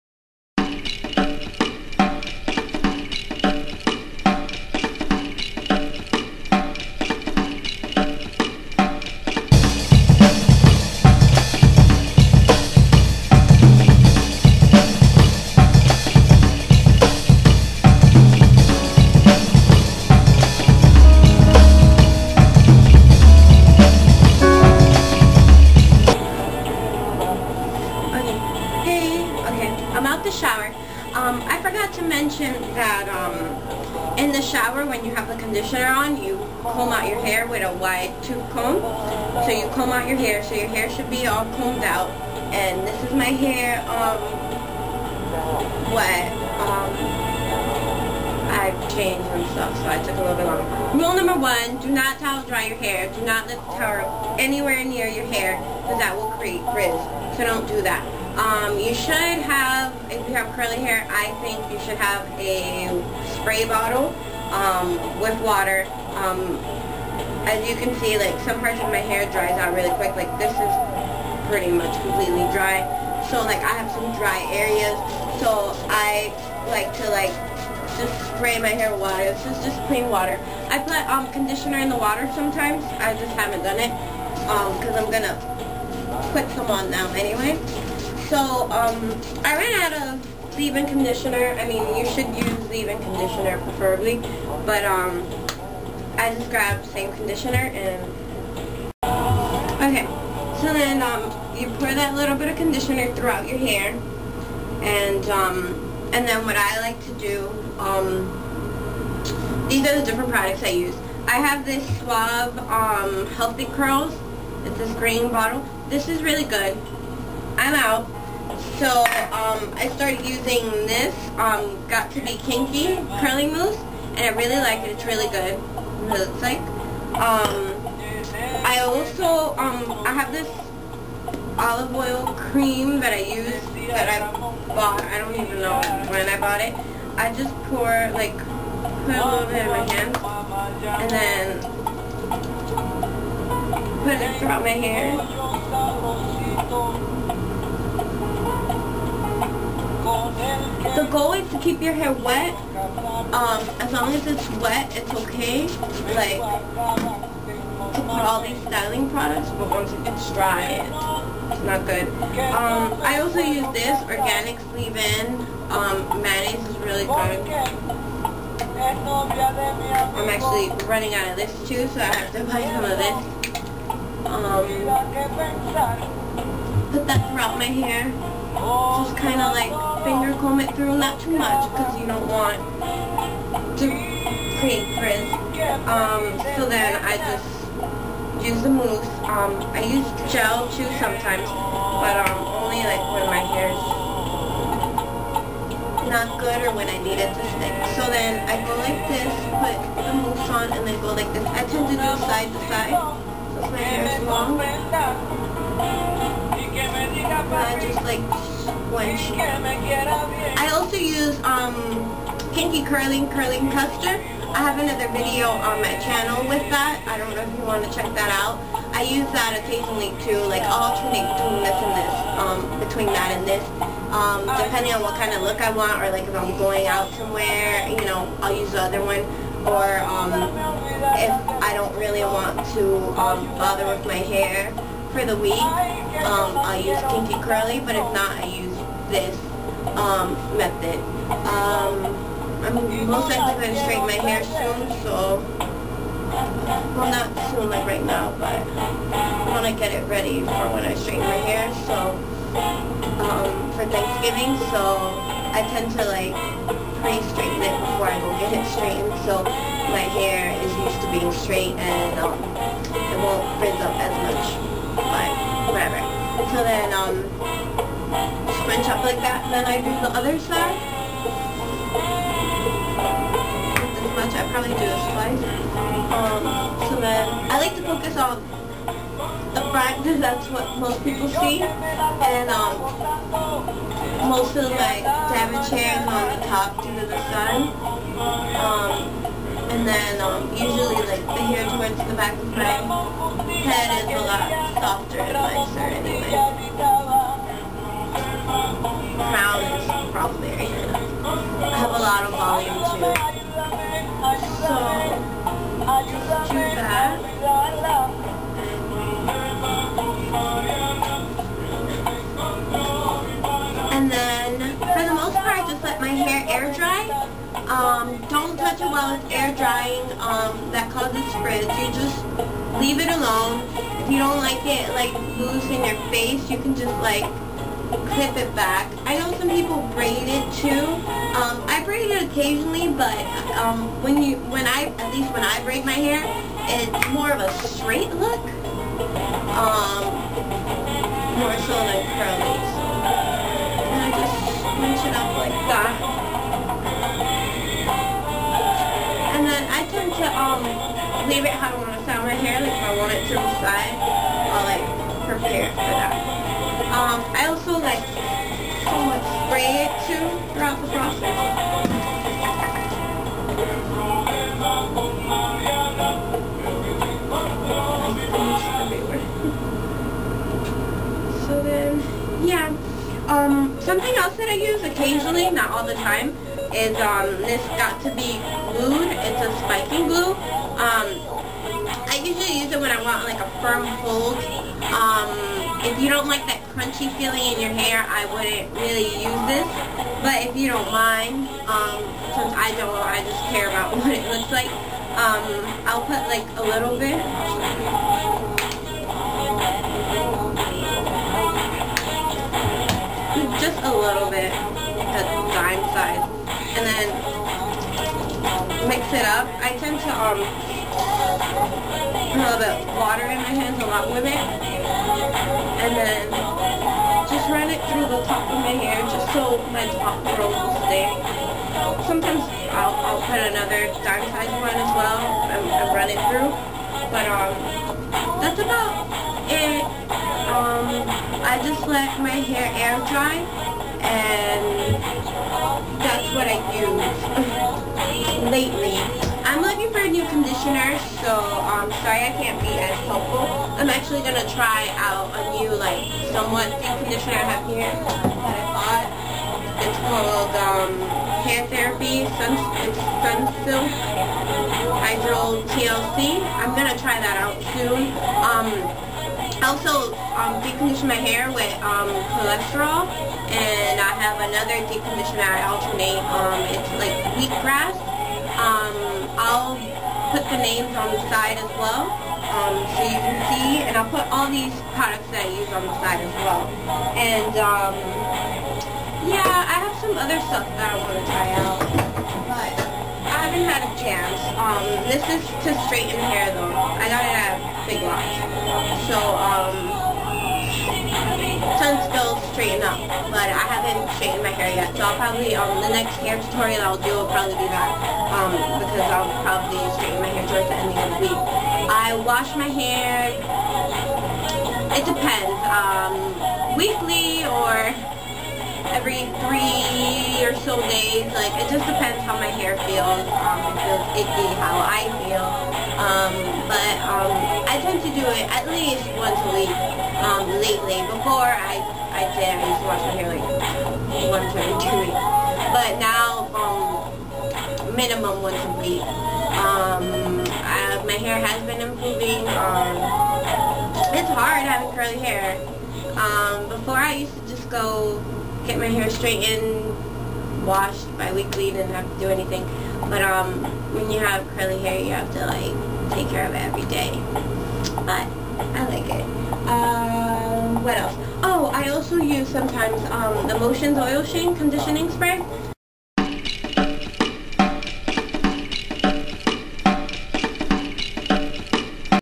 How I style my Hair
A quick tut on how I style my hair
Will update with products soon